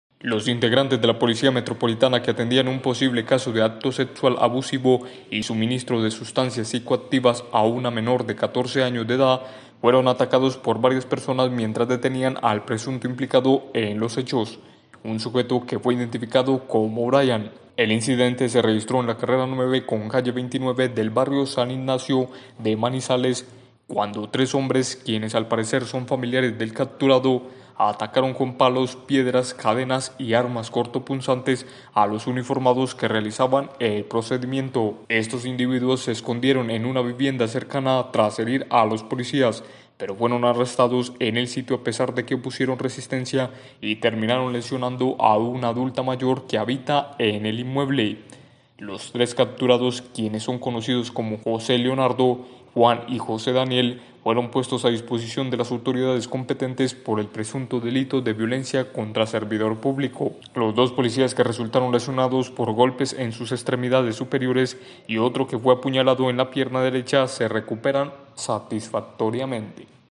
TRES POLICÍAS HERIDOS EN MANIZALES CUANDO REALIZABAN UNA CAPTURA
Los integrantes de la Policía Metropolitana que atendían un posible caso de acto sexual abusivo y suministro de sustancias psicoactivas a una menor de catorce años de edad, fueron atacados por varias personas mientras detenían al presunto implicado en los hechos; un sujeto que fue identificado como 'Brayan'.